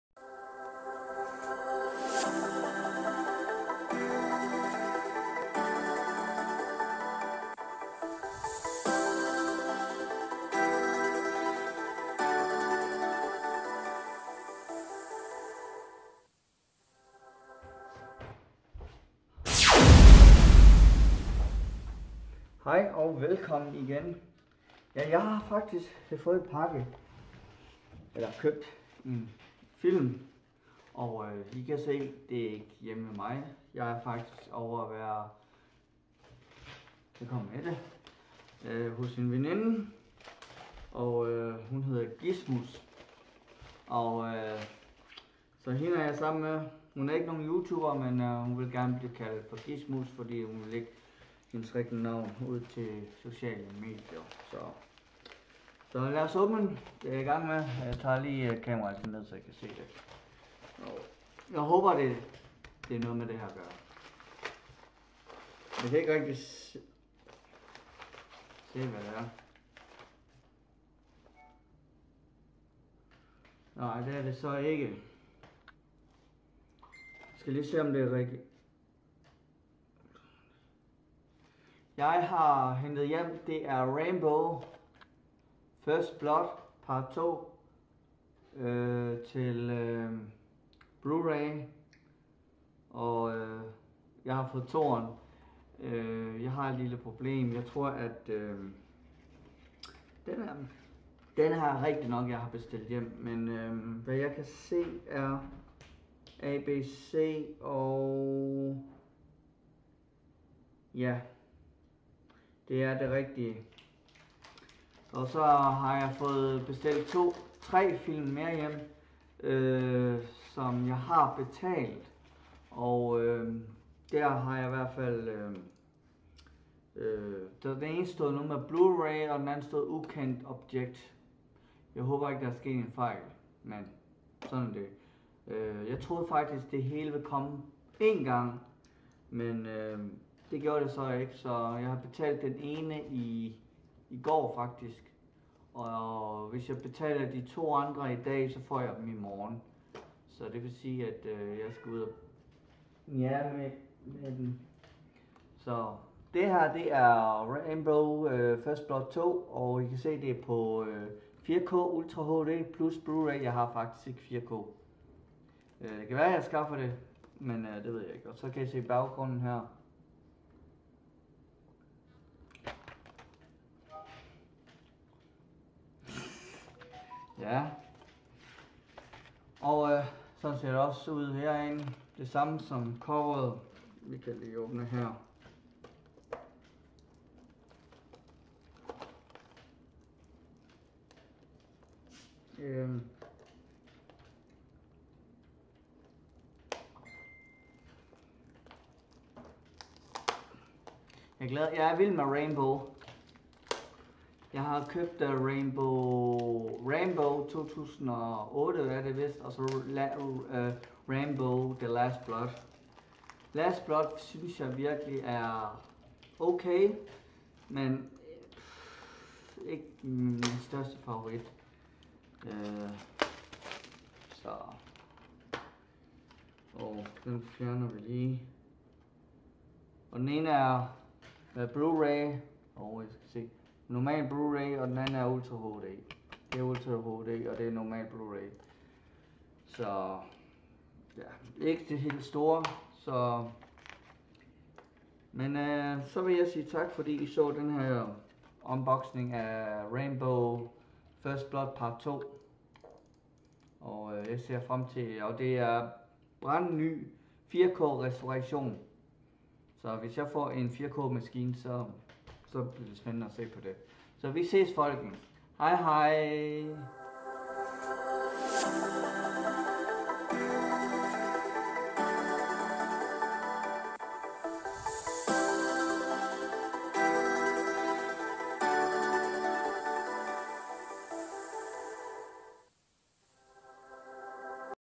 RAMBO: FIRST BLOOD PART II. 4K/BLURAY (Udpakning)
SJUUU udpaknings folkens så jeg tilbage igen med en ny udpaknings video til jer hvor vi ser nærmere en kult klassiker...Rambo First Blood part 2. med vært er Gizmus